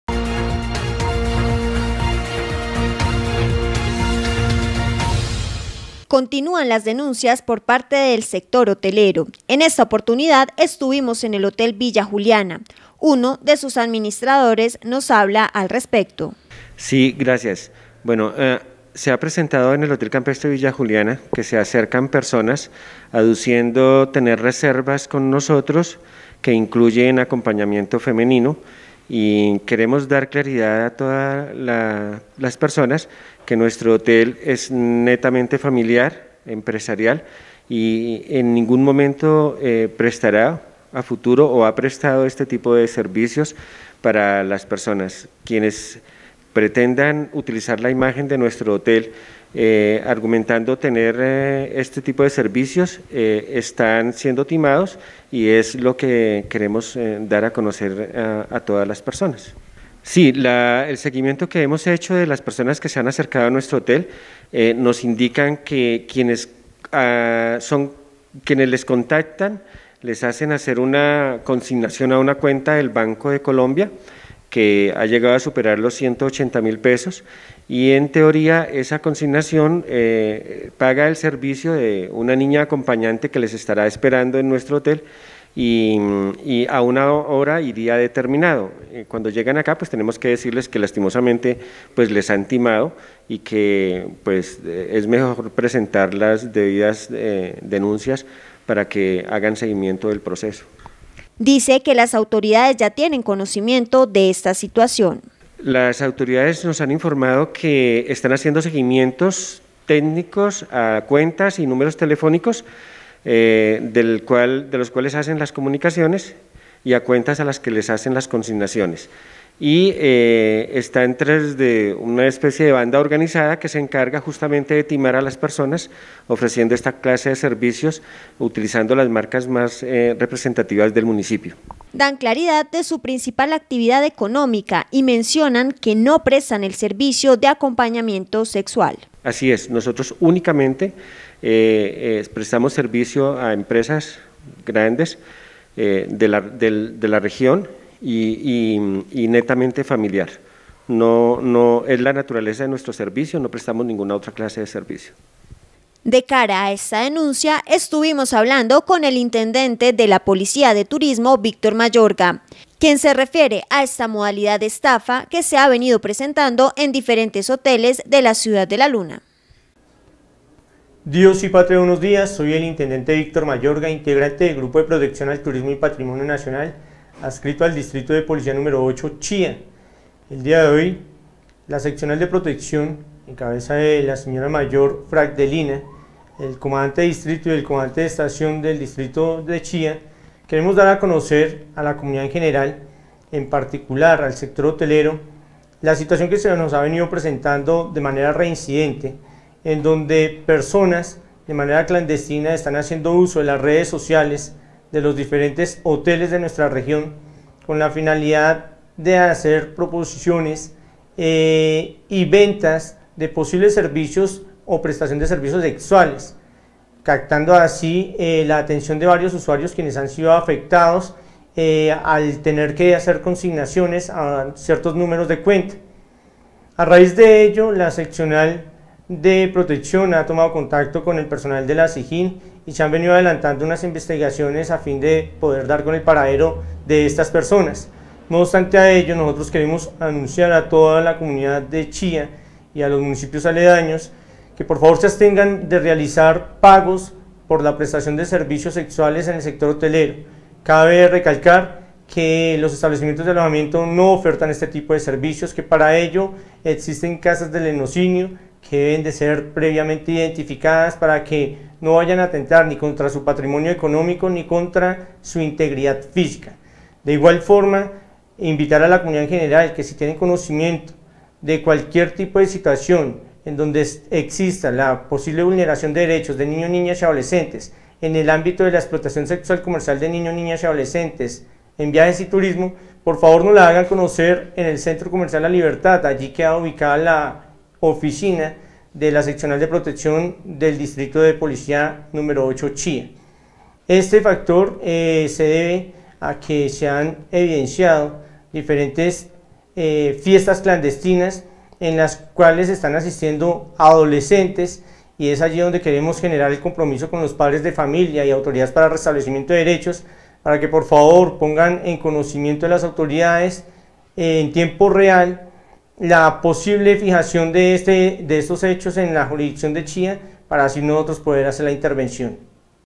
El Hotel Campestre Villa Juliana da a conocer una denuncia ante las cámaras de LTC noticias
Dan claridad sobre su principal actividad económica, así mismo se refiere a la manera de operar de los presuntos delincuentes, dejan claro que esta situación ya está en conocimiento de las autoridades competentes y que están en espera de una respuesta pues ya son varias las personas estafadas.